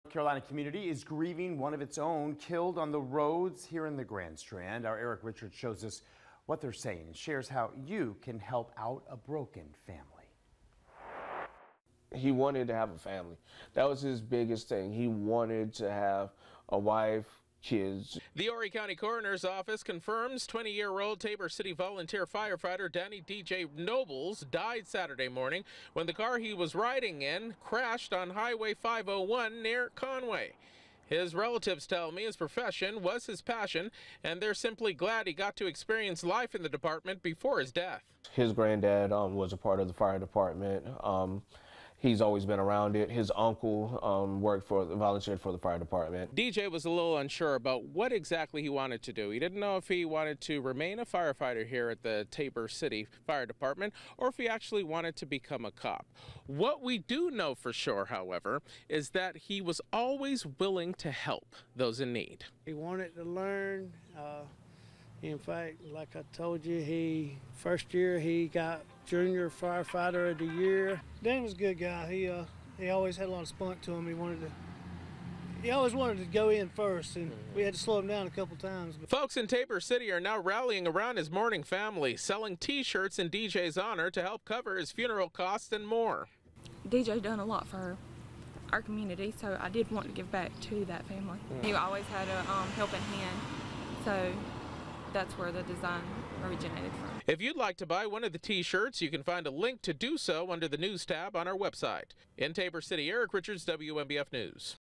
N.C. community remembers firefighter killed in Horry County crash
The small community of Tabor City is coming together to remember a volunteer firefighter killed in a weekend crash.